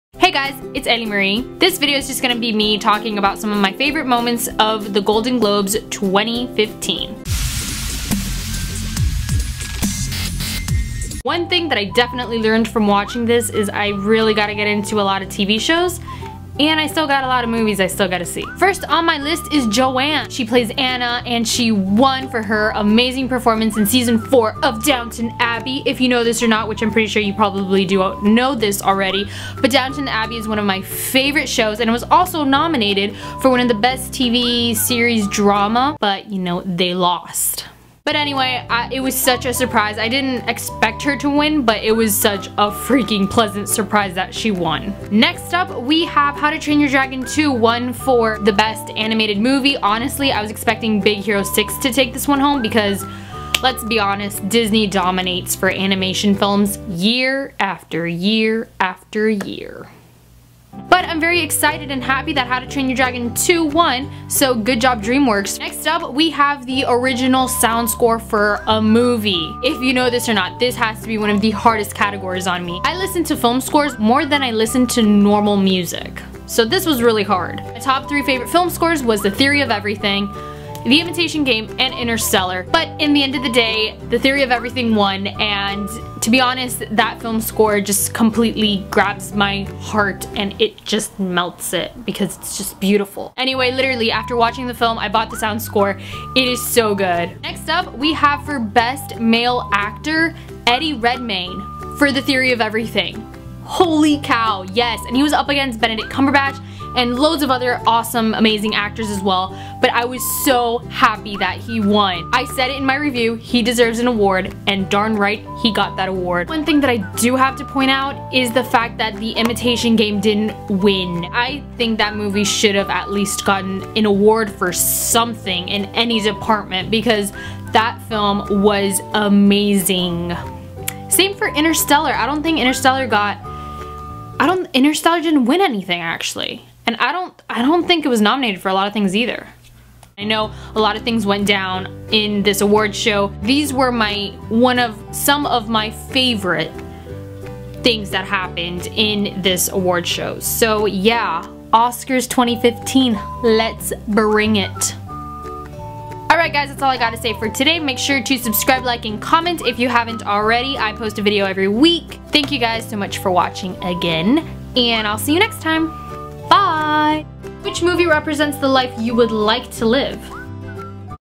Golden Globe Awards 2015 Favorites
I tried to make this a short list my favorite awards given at The Golden Globe Awards 2015. Thus I wasn't able to fit everything I enjoyed...I only listed my top favorite awards.

The Golden Globe Awards of 2015 was great!

Although it's just foreshadowing what will happen in the Oscars. thank you Golden Globes for giving us an idea.

Well I hope you enjoyed my Golden Globe Awards 2015 Favorites Review.